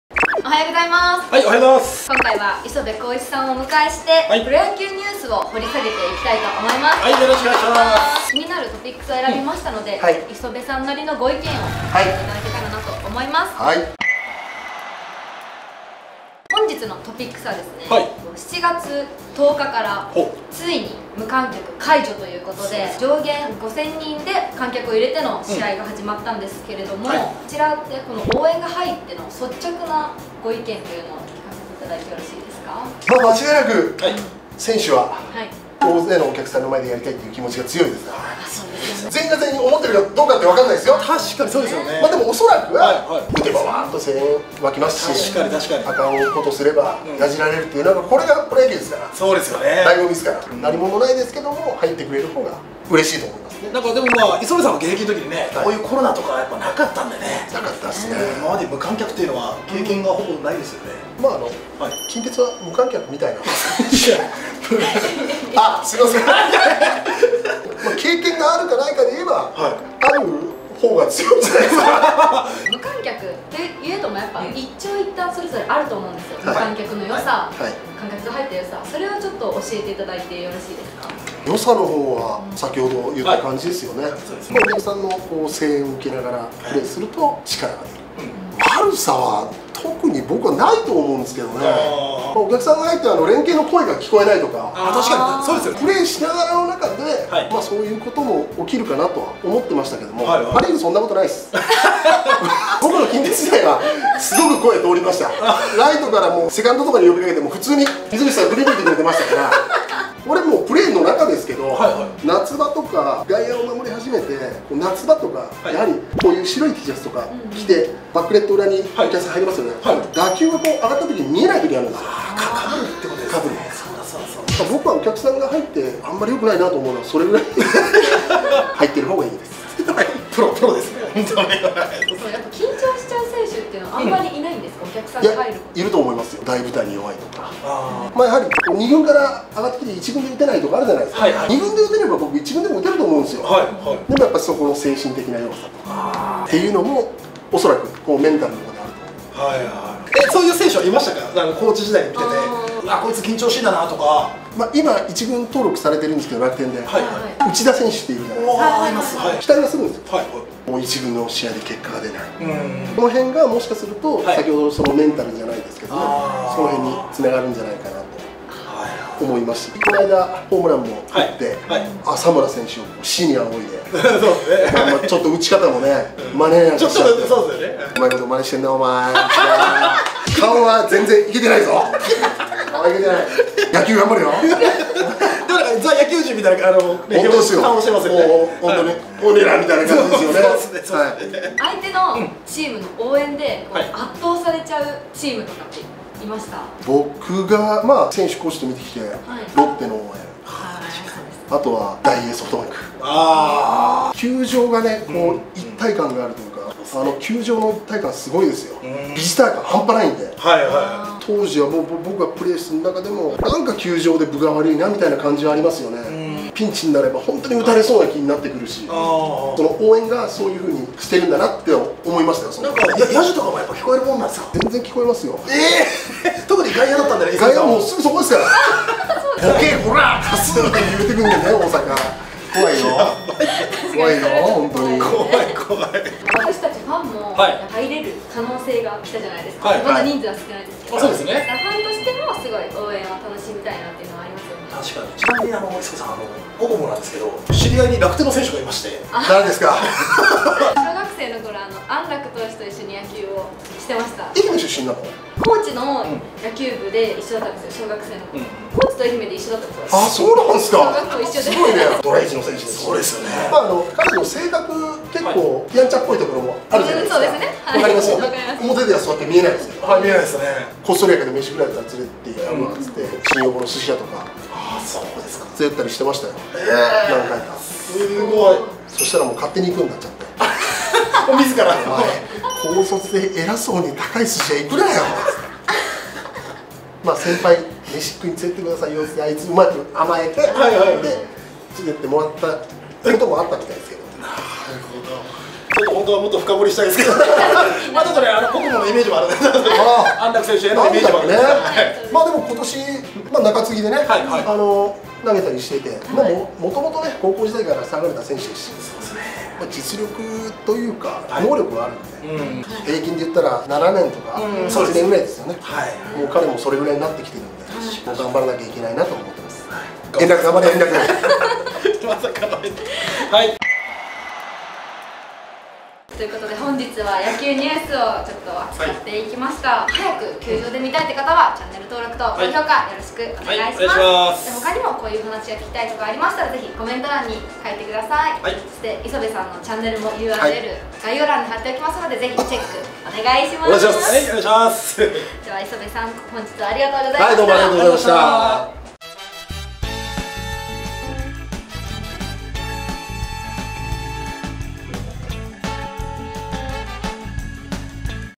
【礒部公一】プロ野球はファンの声援が大事や！元楽天イーグルス・近鉄バファローズ選手会長の礒部公一さんが語る観客動員🔥
本日は、元楽天イーグルスの礒部公一さんい来ていただきました🎉
やはりファンのいる野球は最高ですね😆 皆さんはもう試合観に行きましたか！？
これからも野球を盛り上げていきましょう👏🏻  ぜひご覧ください🤩

◇チェック必須の再生リスト◇

◇サムネイル写真◇
提供　朝日新聞社

◇製作◇
朝日新聞社 / 日刊スポーツ新聞社 / BitStar Studio